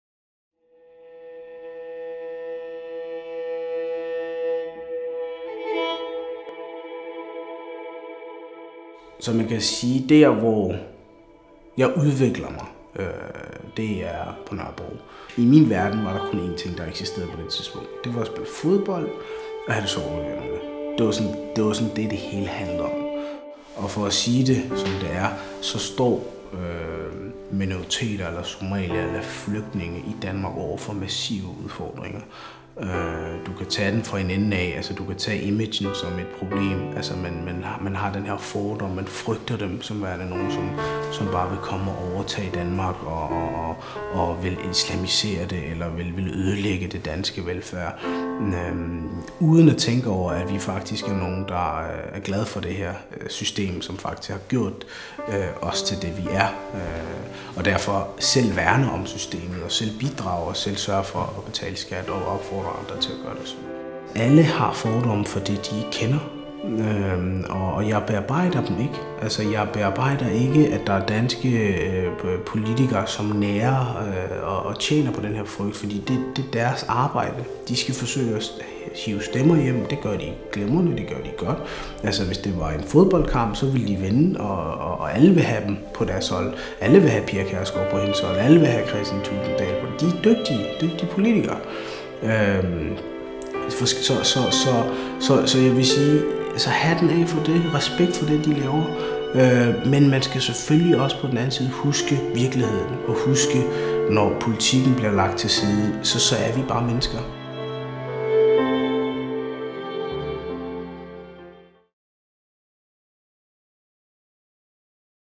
SAID HUSSEIN - LYKKENS GAVE - FORTUNES GIFT
14 positive stories of refugees and ordinary Danish citizens who seek to strengthen our common humanity.